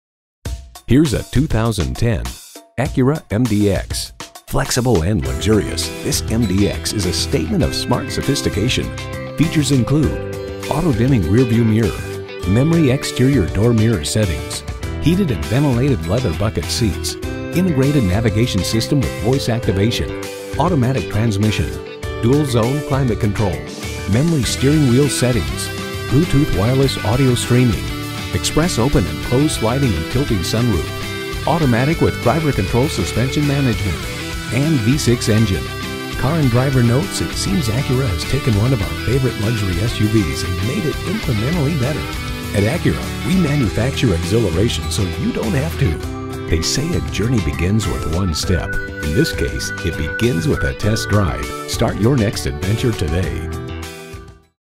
Used 2010 Acura MDX Greenville SC Easley, SC A23043A - SOLD
Year: 2010
Make: Acura
Model: MDX
Trim: 3.7L Advance Package
Engine: 3.7L V6 SOHC VTEC 24V
Transmission: 6-Speed Automatic
Color: Black
Interior: Ebony
Mileage: 104576
Stock #: A23043A
VIN: 2HNYD2H71AH501099

Address:
2450 Laurens Rd